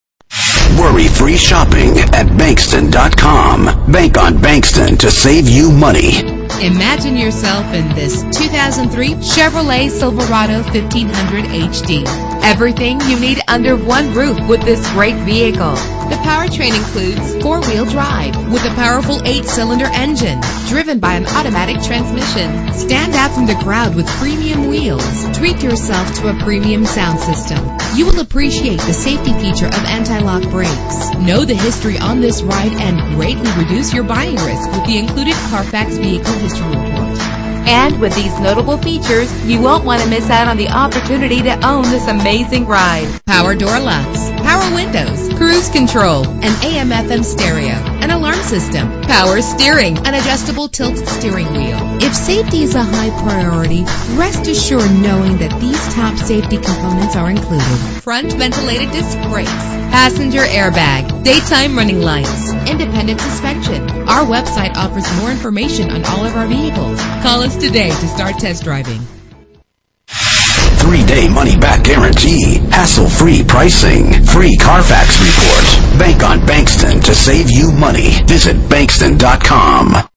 2003 Chevrolet Silverado 1500HD - Dallas TX
This vehicle represents similar vehicles available at Bankston Chevrolet (Autonation).
for availability of specific vehicles.
Phone:
Year: 2003
Make: Chevrolet
Model: Silverado 1500HD
Trim: LT
Engine: 6.0 liter 8 cylinder 16 valve
Transmission: 4-Speed Automatic
Color: Black
Mileage: 86625
Address:
4747 LBJ Freeway